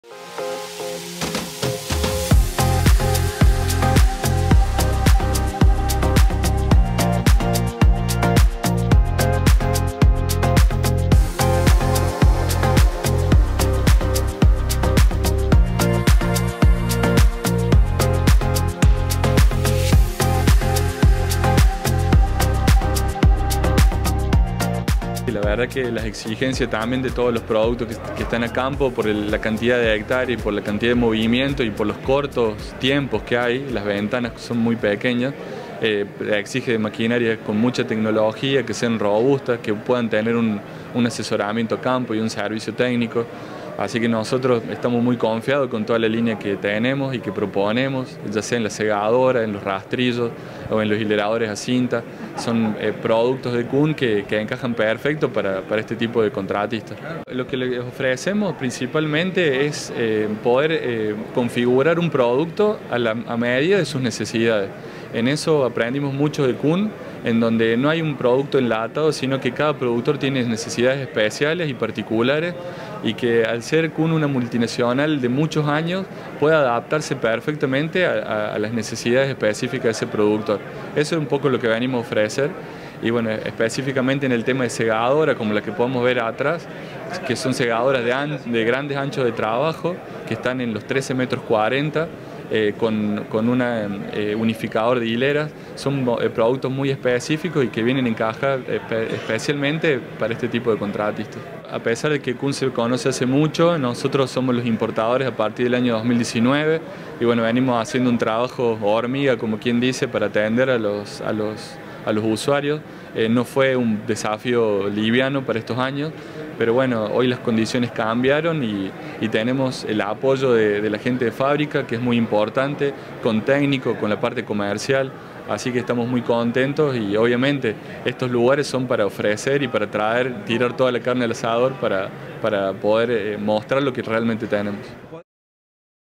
De la mano de Lancioni, Kuhn mostró su oferta en la Plenaria de la CACF
Proveedores de Contratistas Forrajeros ante el desafío de una nueva y complicada campaña